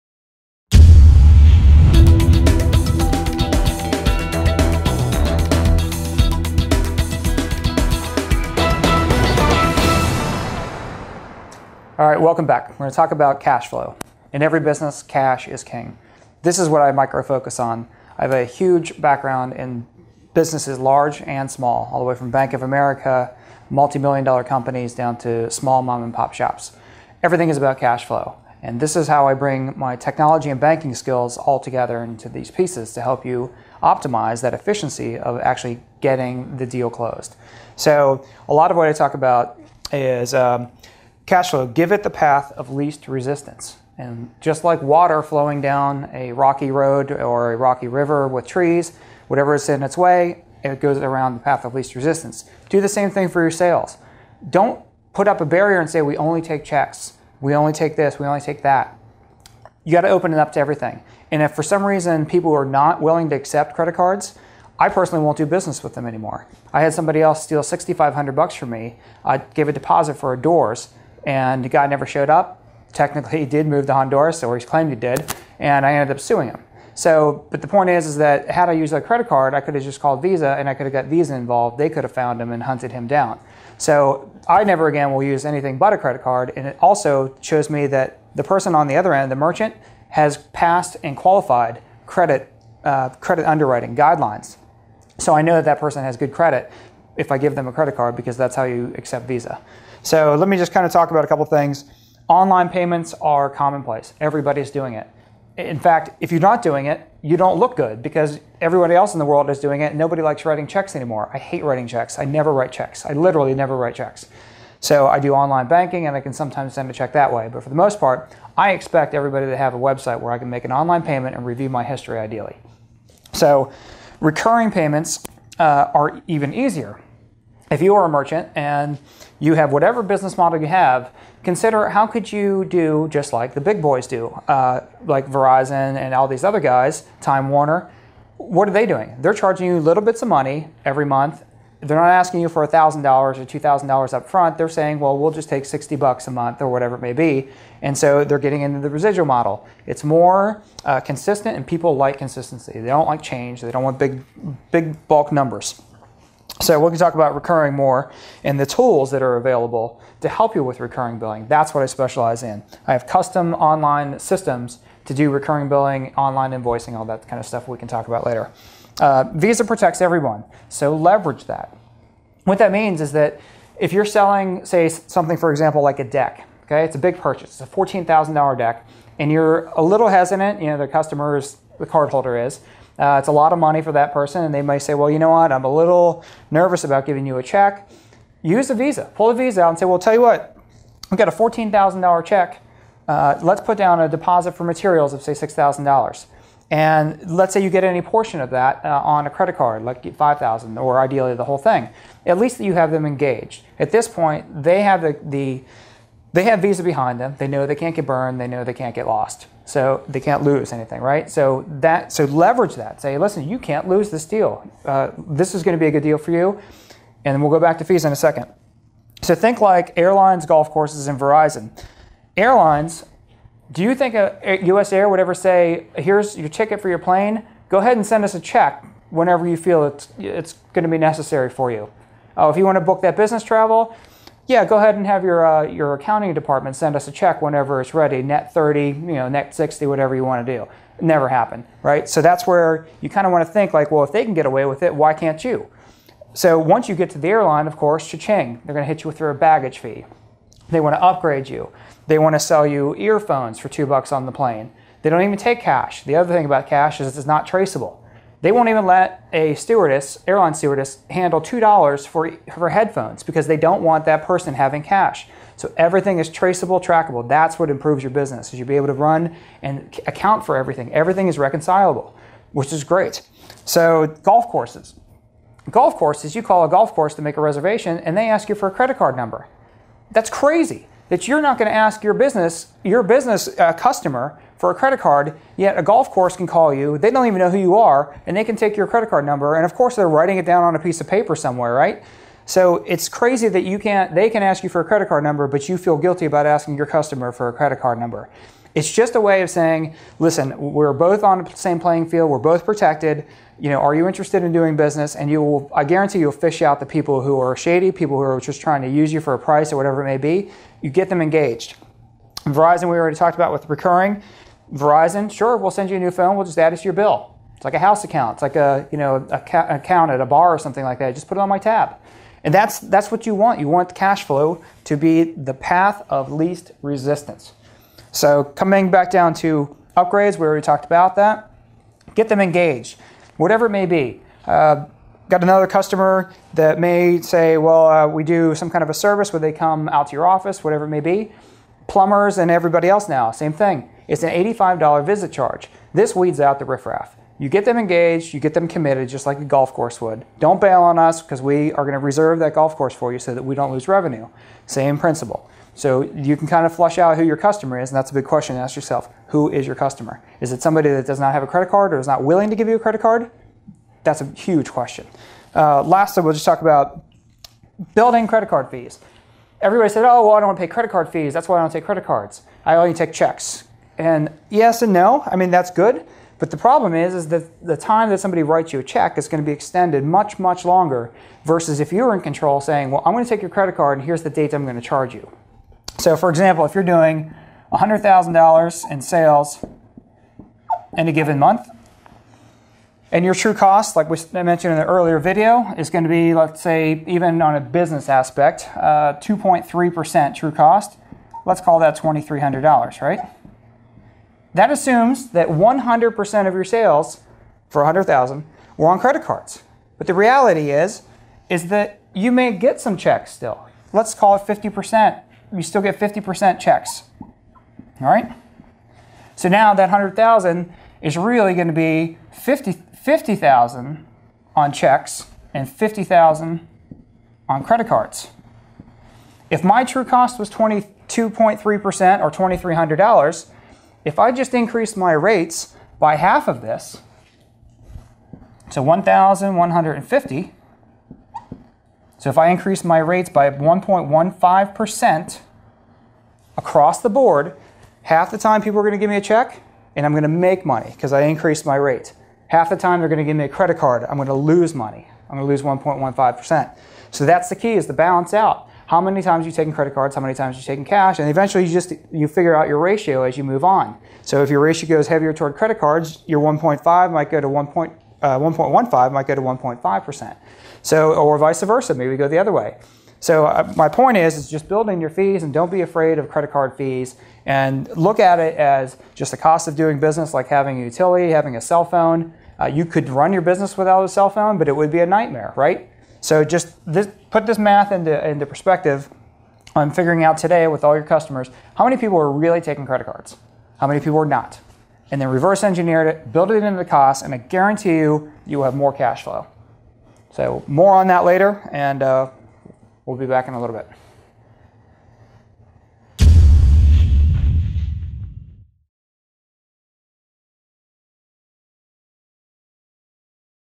Straightline Processing - Cash Flow & How Credit Cards Can Greatly Enhance Your Business
This video discusses the vital aspect of having good cash flow in your business.  Credit card keep everyone honest, engaged and most importantly, helps you determine who your customer really is!